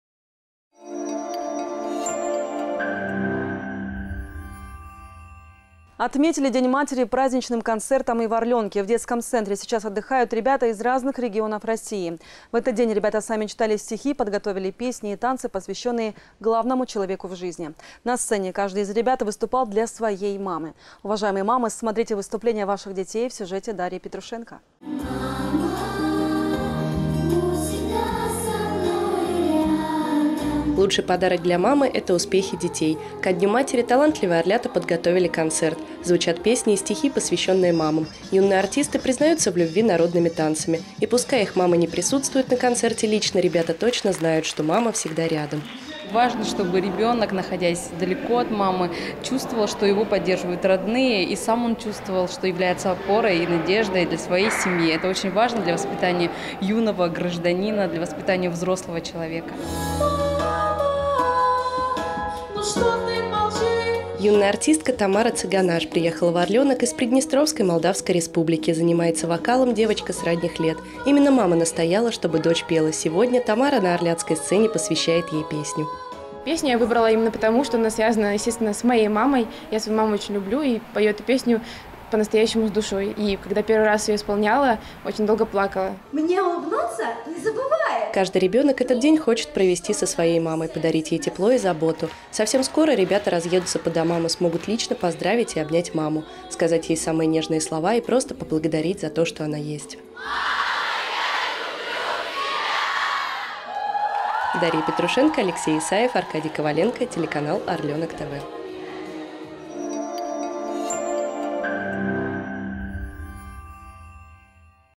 День матери в «Орлёнке»
В «Орлёнке» отметили самый главный праздник - День матери. На праздничном концерте мальчишки и девчонки выступили для своих мам.
Корреспондент:  Дарья Петрушенко
Оператор: Алексей Исаев
Монтаж: Аркадий Коваленко